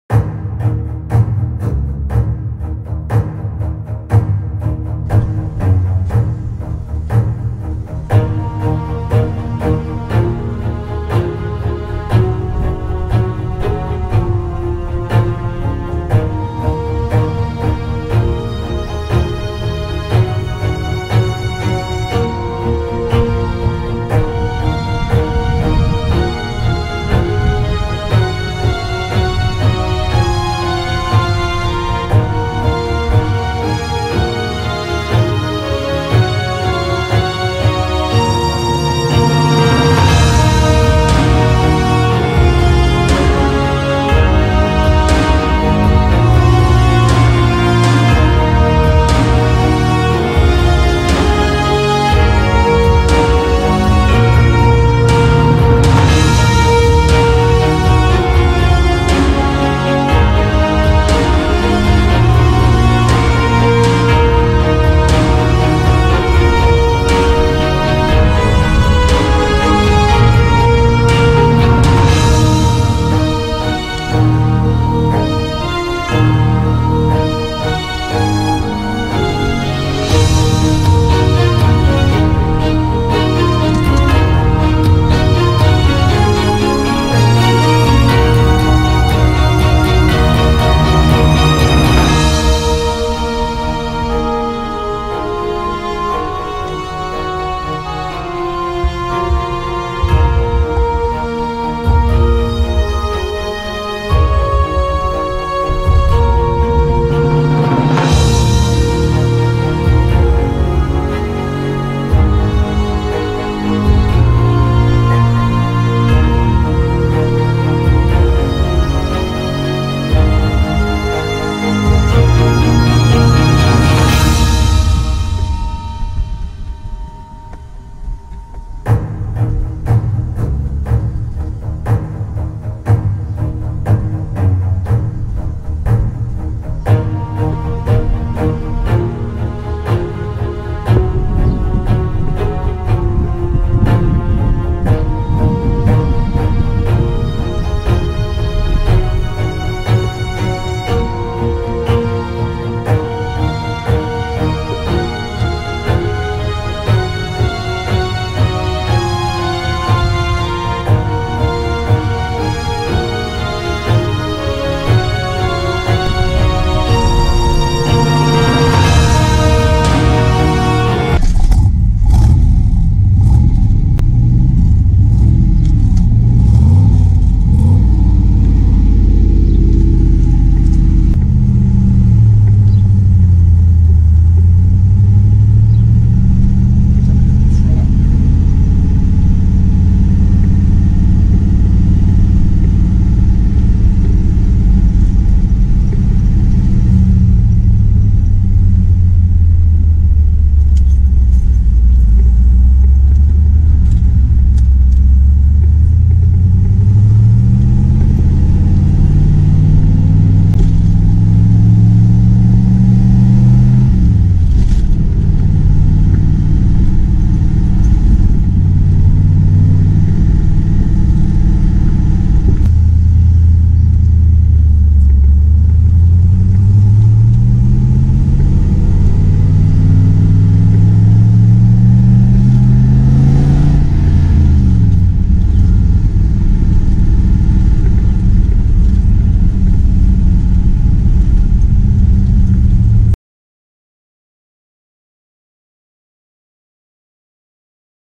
1967 Chevrolet Corvette Convertible 427/390 w/2 tops
Nut and bolt restoration with minimal road use since completion. 427/390 with both hard and soft tops.